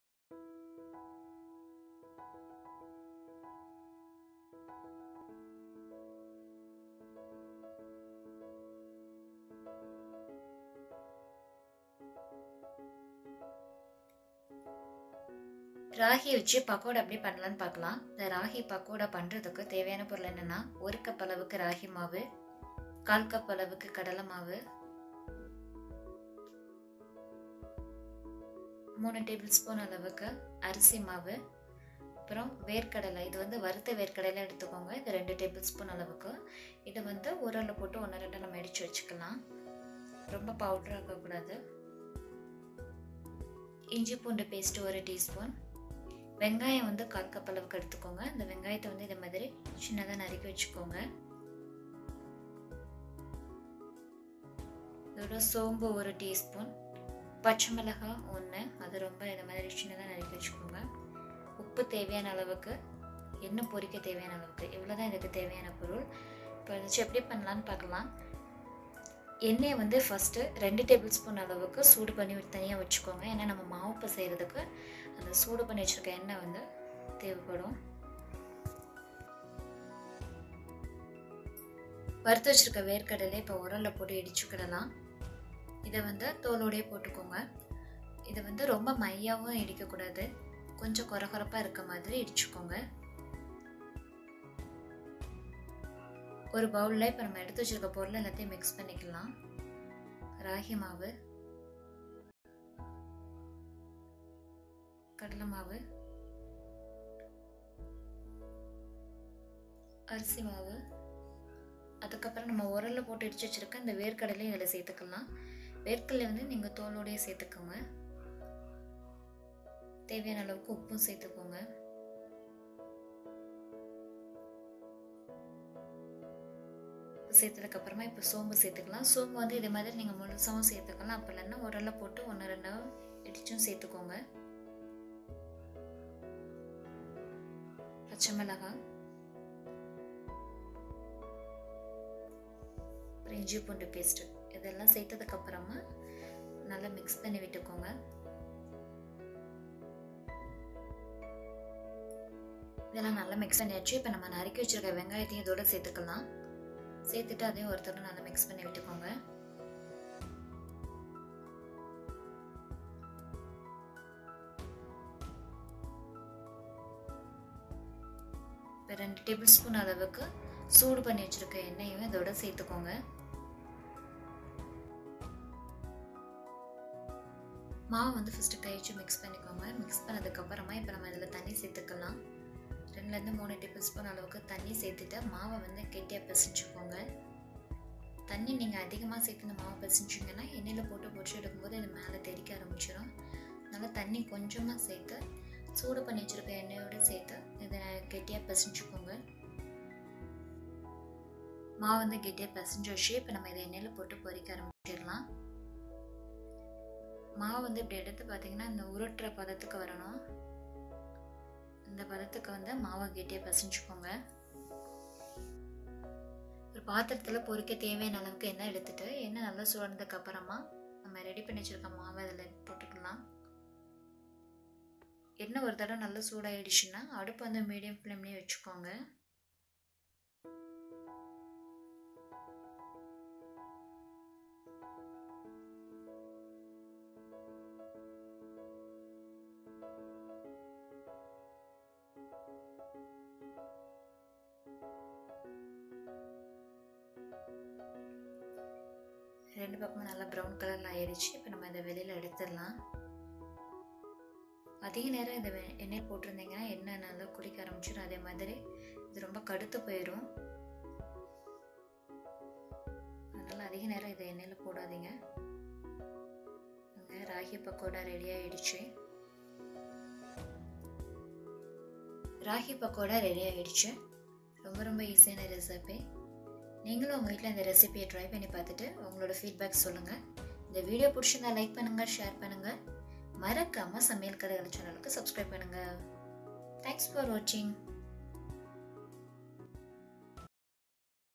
ராகி பக்கோடா Ragi Pakoda Recipe in Tamil | Finger milet recipe | Healthy snacks
Ingredients :
1.Ragi flour - 1 cup
2.Rice flour - 3 tbsp
3.Besan flour -1/4 cup
4.Onion(chopped) - 1/4 cup
5.Green chili - 1
6.Salt - to taste
7.Fennel seeds - 1tsp
8.Peanuts (roasted) - 2tbsp
9.Hot oil - 2 tbsp
10.Water -  3 tbsp

Please provide your feedback in comment section below.